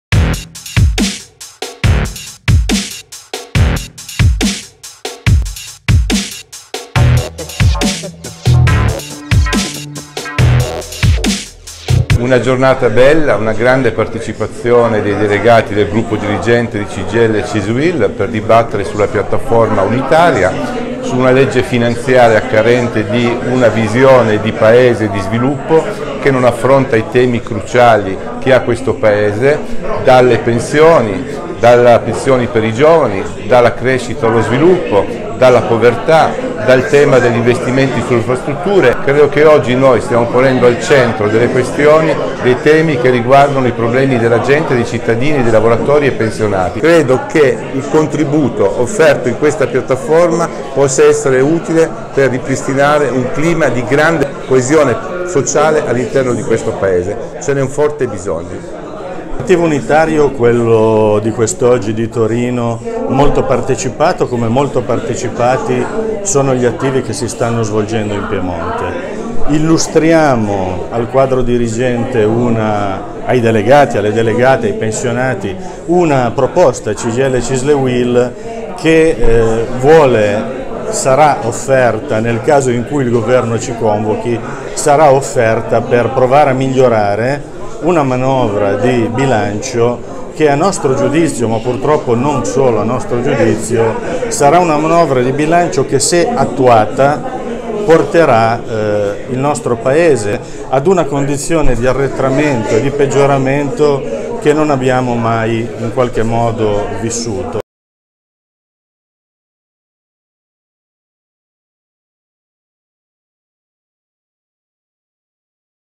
Attivo unitario Cgil Cisl Uil Torino: parlano i segretari Cisl Lo Bianco e Ferraris
Il commento dei segretari territoriale e regionale Cisl, Domenico Lo Bianco e Alessio Ferraris, sulla Legge di Bilancio  al centro dell’Attivo unitario di Cgil Cisl Uil Torino del 23 novembre 2018